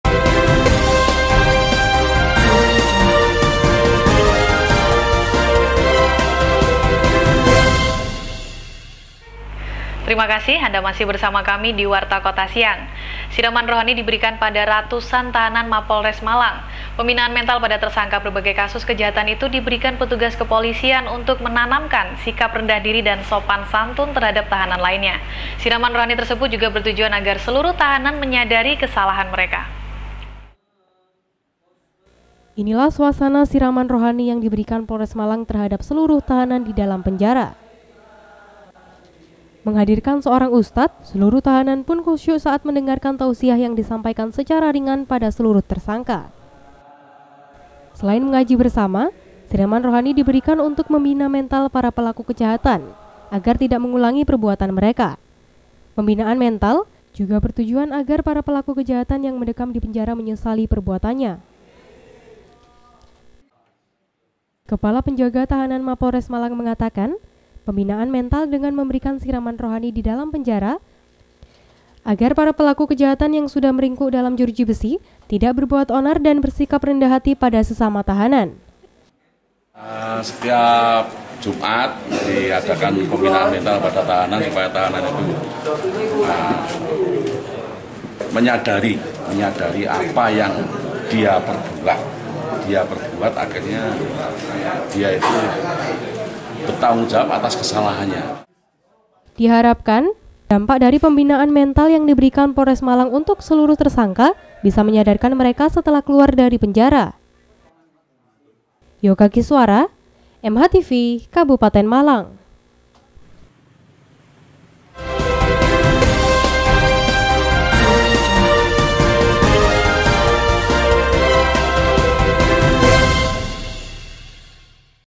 KABUPATEN MALANG - PULUHAN TAHANAN POLRES MALANG DIBERI SIRAMAN ROHANI
Siraman Rohani diberikan pada ratusan tahanan Mapolres Malang. Pembinaan mental pada tersangka berbagai kasus kejahatan tersebut diberikan petugas kepolisian untuk menanamkan sikap rendah diri dan sopan santun terhadap tahanan lainya.